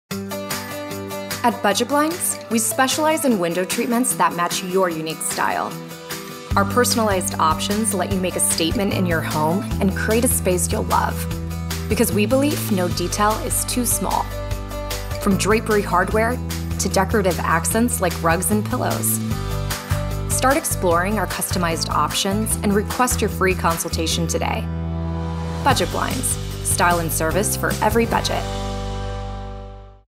Customization | Budget Blinds
Since window treatments are such a huge part of how you feel about your home, why should you settle for something that's not perfectly tailored to you? With customized window treatments, layered draperies, and decorative accents, Budget Blinds allows you to make a unique statement in your home.

Explore all our custom options at BudgetBlinds.com.